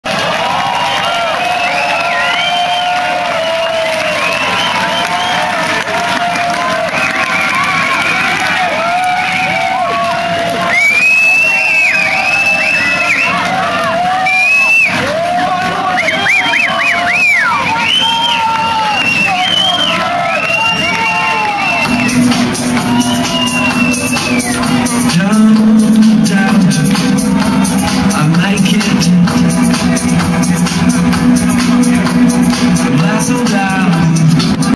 Digweed Fabric 10 Feb 2007 - CLIP 10 - 8:03am
The end?  Round of applause at 8am, before going into a beautiful deep chilled vocal track